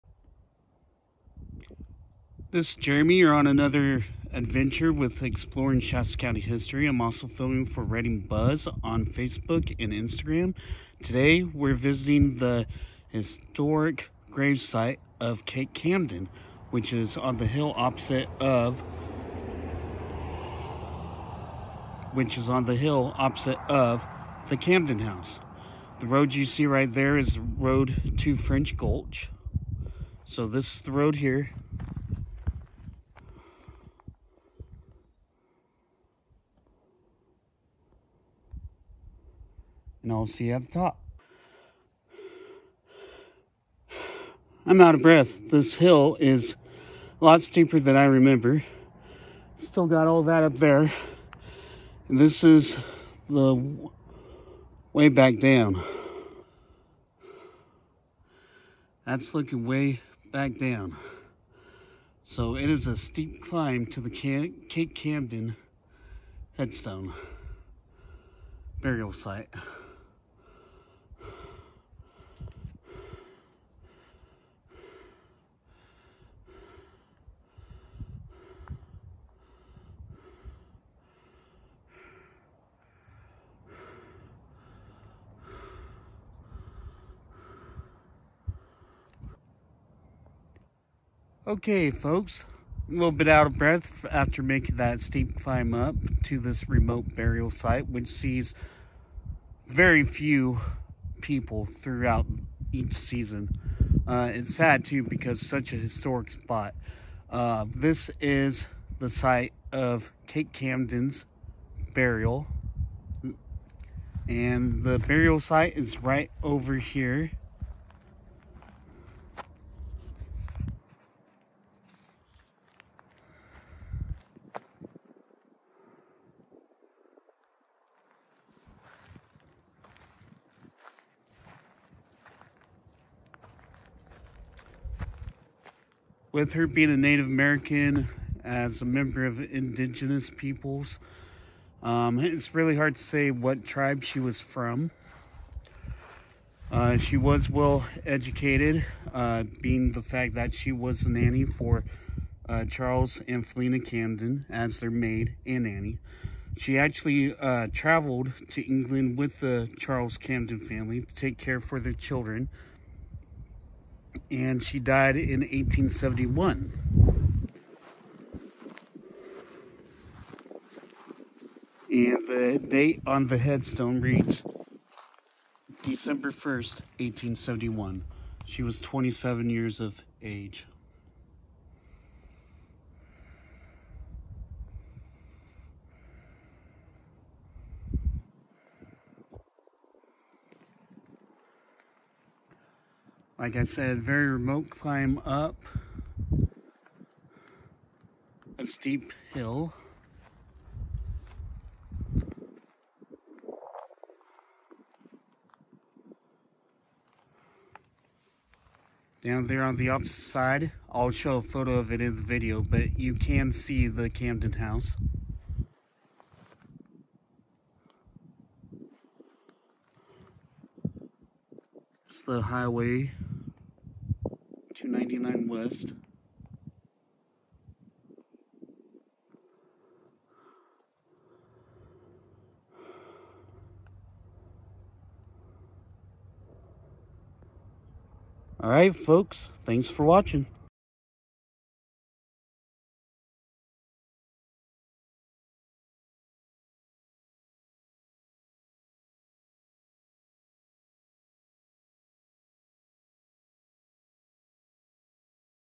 THE BURIAL SITE OF KATE CAMDEN.
On this episode of Exploring Shasta County history, Jeremy takes you on an adventure to the historic burial site of Kate Camden. Learn more about her in the video. Enjoy!